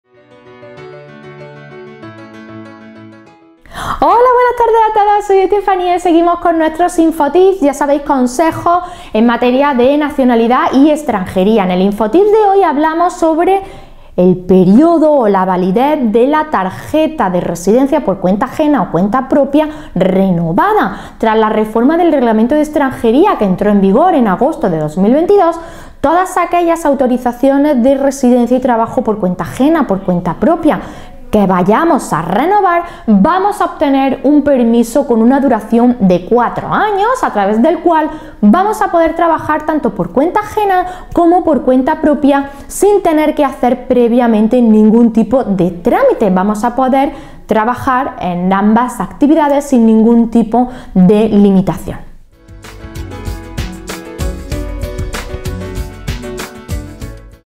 💡 InfoTips: Duración de las Renovaciones de Residencia 💡
💡 Hoy en nuestros InfoTips, consejos útiles para tus trámites de extranjería y nacionalidad española 🙋🏻‍♂️ hablamos sobre la Duración de las Renovaciones de Residencia ➡️